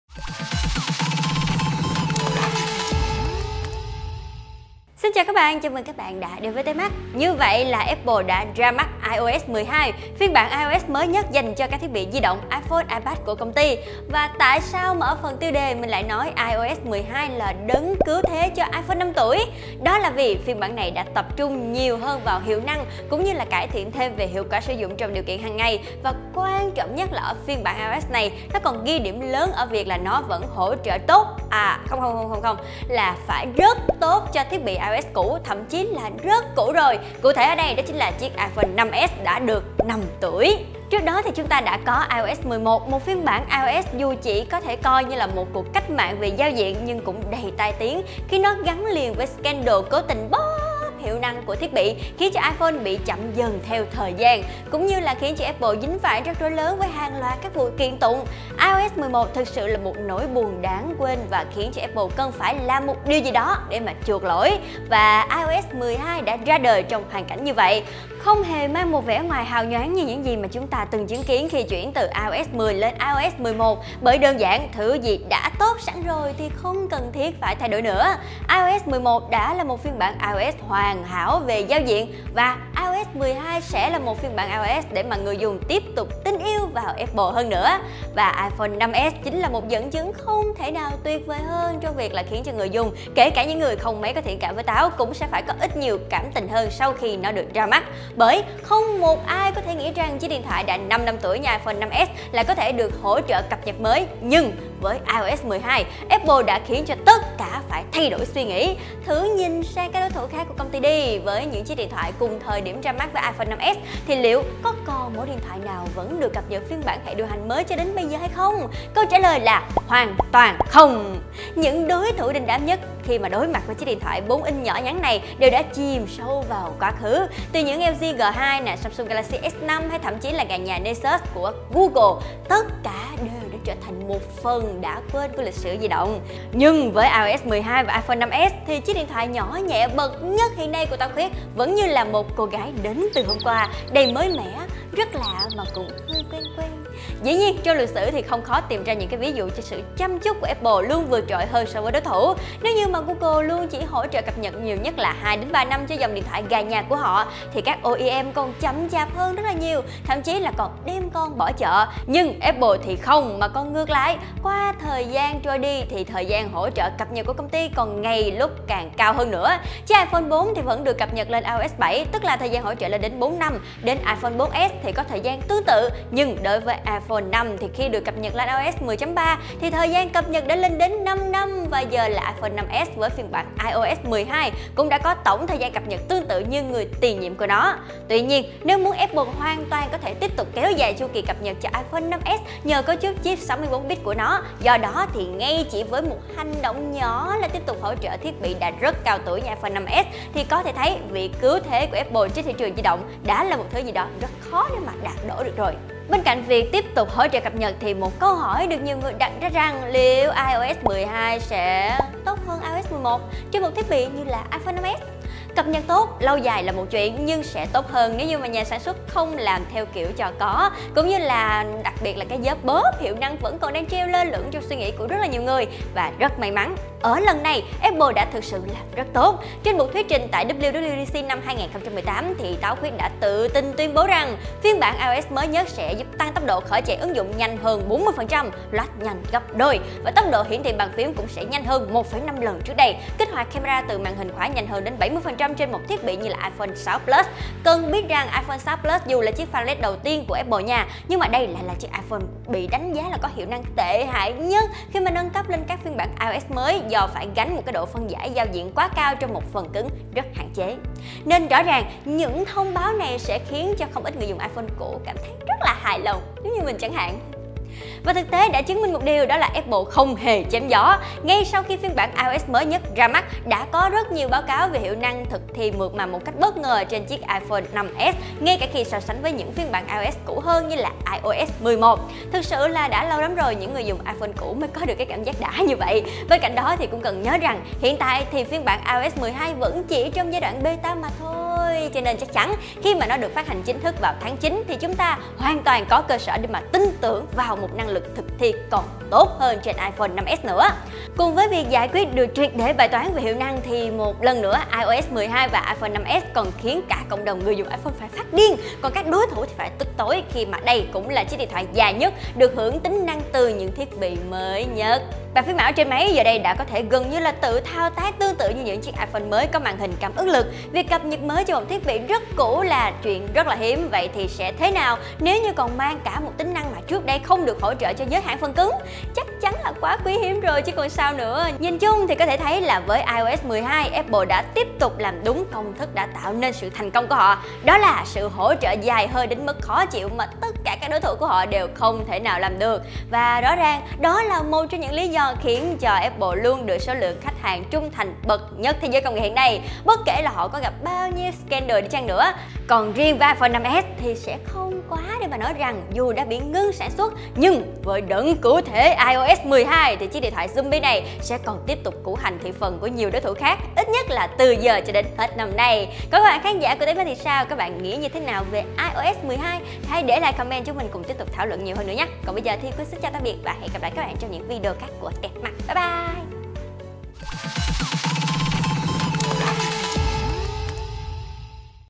iOS 12 - "Đấng cứu thế" cho iPhone 5 năm tuổi…
Apple - từ trước đến nay đã luôn được coi là một kẻ đặc biệt trong thế giới công nghệ. Không chỉ thành công trong sự nổi tiếng với các thiết bị iOS, áp đặt một cách hoàn hảo sự ảnh hưởng của từng đừng nét thiết kế trên chiếc iPhone lên hầu hết đối thủ trên thị trường thị động, hay vẫn “một mình một ngựa” ăn gần như sạch sẽ lợi nhuận, hoặc thậm chí là sống vui, sống khỏe với những chiếc iPad mặc cho thế giới tablet đang ngày một suy vong, mà điều khiến Apple luôn được các ifan yêu quý một cách “cuồng nhiệt” - chính là thời gian hỗ trợ cập nhật phần mềm mới cực kỳ dài, mà hầu như ngoài Táo khuyết, không có bất cứ công ty phần cứng nào khác đủ tầm làm được.